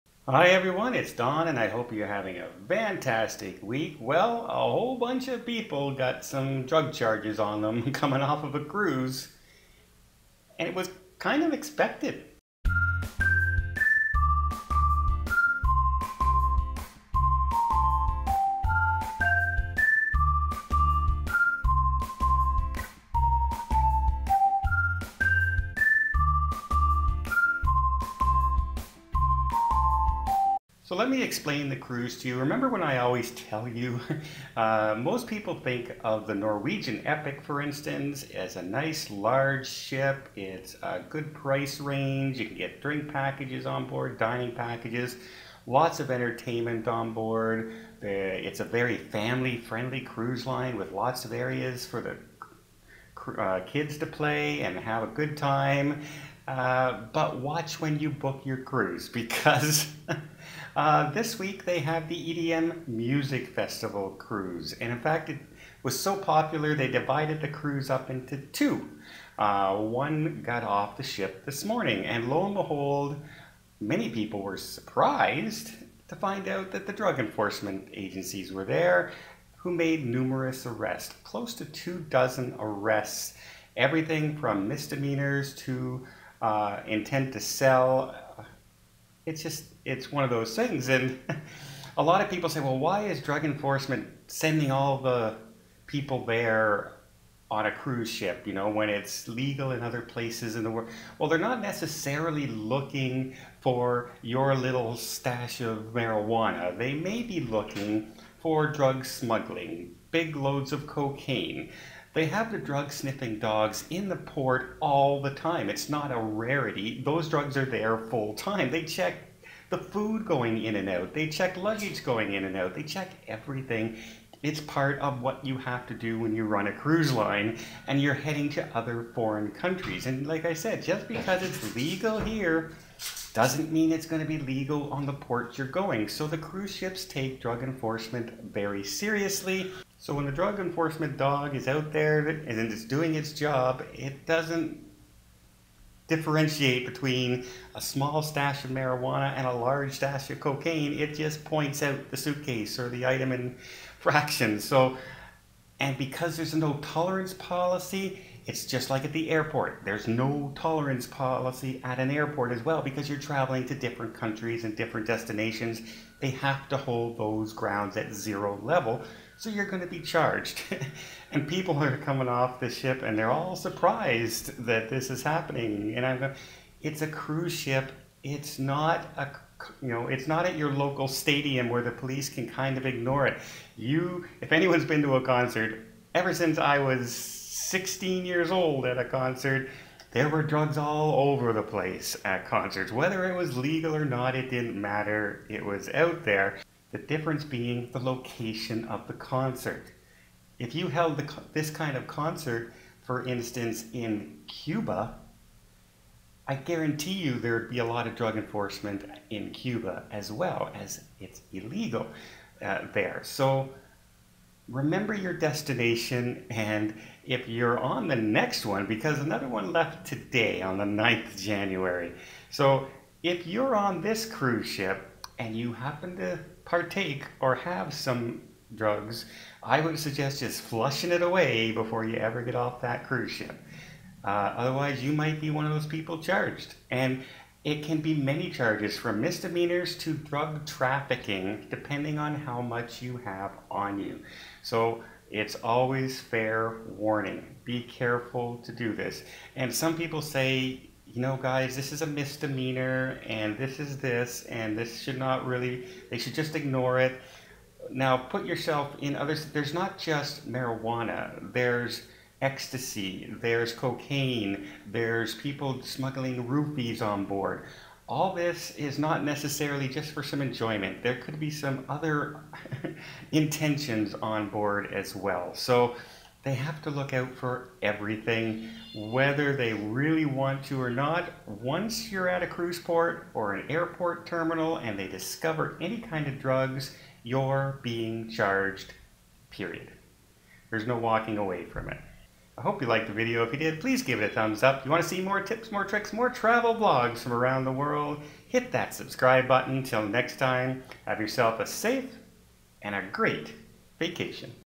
Passengers charged getting off Norwegian cruise
Passengers charged getting off Norwegian cruise. People need to realize a cruise ship and going through customs at airports and cruise terminals have to take things very seriously.  Going to a music festival at your local park is not the same as going to one held on a cruise ship sailing into international waters.  Here's why.